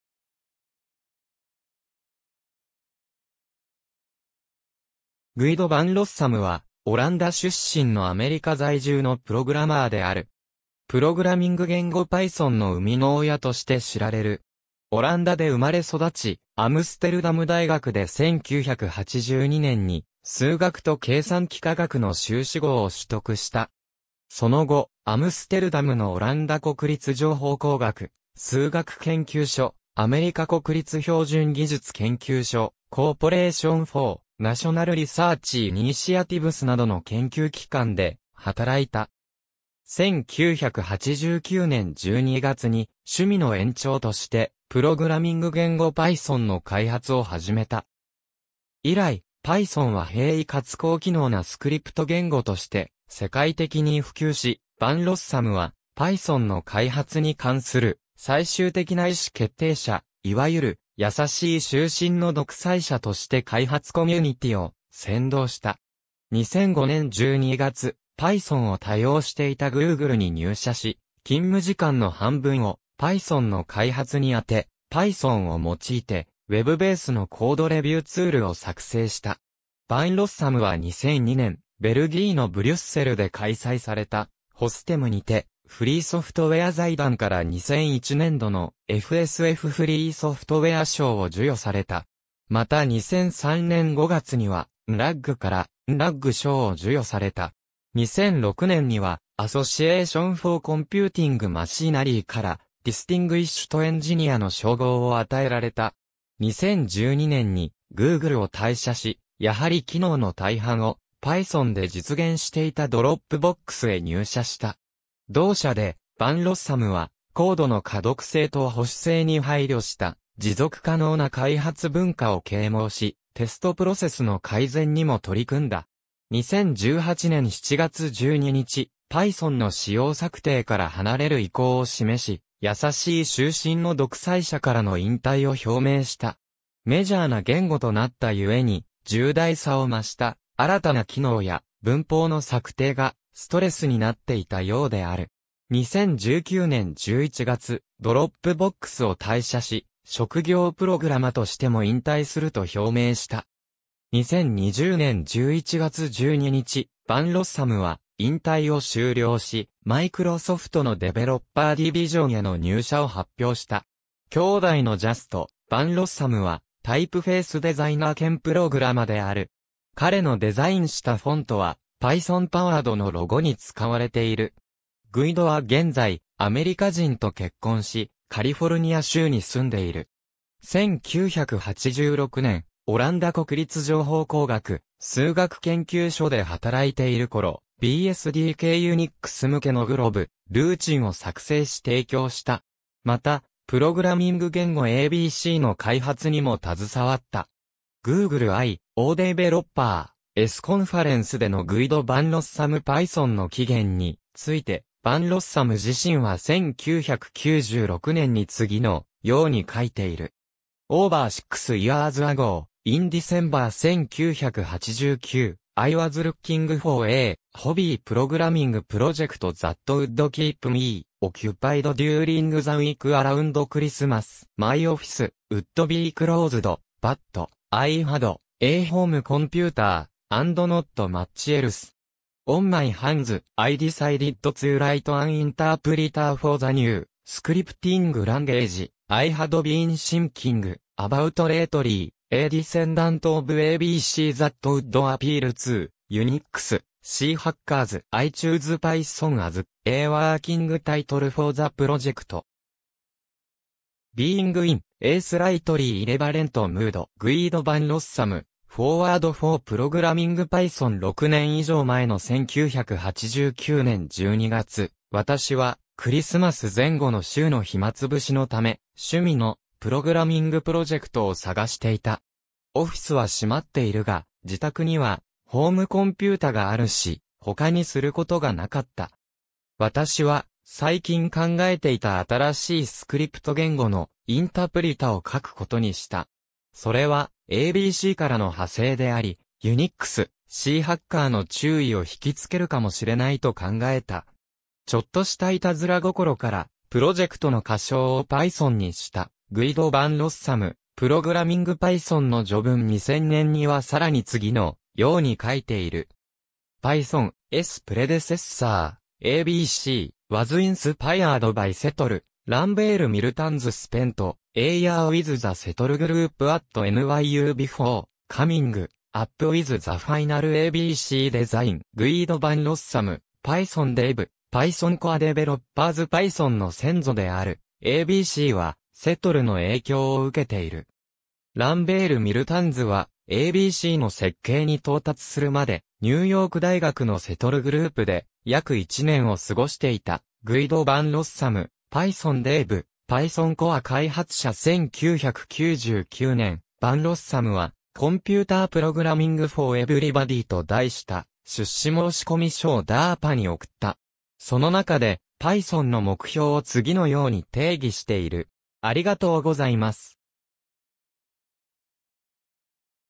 グイド・ヴァンロッサム
グイド・ヴァンロッサム（Guido van Rossum、1956年1月31日 - ）はオランダ出身のアメリカ在住のプログラマーである。
プログラミング言語Pythonの生みの親として知られる。
オランダで生まれ育ち、アムステルダム大学で1982年に数学と計算機科学の修士号を取得した。
その後、アムステルダムのオランダ国立情報工学・数学研究所 (CWI)、アメリカ国立標準技術研究所 (NIST)、Corporation for National Research Initiatives (CNRI) などの研究機関で働いた。
1989年12月に趣味の延長としてプログラミング言語 Python の開発を始めた。
以来、Pythonは平易かつ高機能なスクリプト言語として世界的に普及し、ヴァンロッサムは Pythonの開発に関する最終的な意思決定者、いわゆる「優しい終身の独裁者（BDFL）」として開発コミュニティを先導した。
2005年12月、Pythonを多用していたGoogleに入社し、勤務時間の半分をPythonの開発に充て、Pythonを用いてウェブベースのコードレビューツールを作成した。
ヴァンロッサムは2002年、ベルギーのブリュッセルで開催されたFOSDEMにて、フリーソフトウェア財団から2001年度のFSFフリーソフトウェア賞を授与された。
また2003年5月にはNLUUG（オランダのUnix/Linuxユーザーグループ）からNLUUG賞を授与された。
2006年には Association for Computing Machinery（ACM）から Distinguished Engineer の称号を与えられた。
2012年にGoogleを退社し、やはり機能の大半をPythonで実現していたDropboxへ入社した。
同社でヴァンロッサムは、コードの可読性と保守性に配慮した持続可能な開発文化を啓蒙し、テストプロセスの改善にも取り組んだ。
2018年7月12日、Pythonの仕様策定から離れる意向を示し、優しい終身の独裁者（BDFL）からの引退を表明した。
メジャーな言語となったゆえに重大さを増した、新たな機能や文法の策定がストレスになっていたようである。
2019年11月、Dropboxを退社し、職業プログラマとしても引退すると表明した。
2020年11月12日、ヴァンロッサムは引退を終了し、マイクロソフトのDeveloper Division（開発者部門）への入社を発表した。
兄弟のJust van Rossumはタイプフェースデザイナー兼プログラマである。
彼のデザインしたフォントは「Python Powered」のロゴに使われている。
グイドは現在、アメリカ人と結婚し、カリフォルニア州に住んでいる。
1986年、オランダ国立情報工学・数学研究所 (CWI) で働いているころ、BSD系UNIX向けのglob()ルーチンを作成し提供した。
また、プログラミング言語ABCの開発にも携わった。
Google I/O Developer's Conference でのグイド・ヴァンロッサム Pythonの起源について、ヴァンロッサム自身は1996年に次のように書いている。
Over six years ago, in December 1989, I was looking for a "hobby" programming project that would keep me occupied during the week around Christmas. My office … would be closed, but I had a home computer, and not much else
on my hands. I decided to write an interpreter for the new scripting language I had been thinking about lately: a descendant of ABC that would appeal to Unix/C hackers. I chose Python as a working title...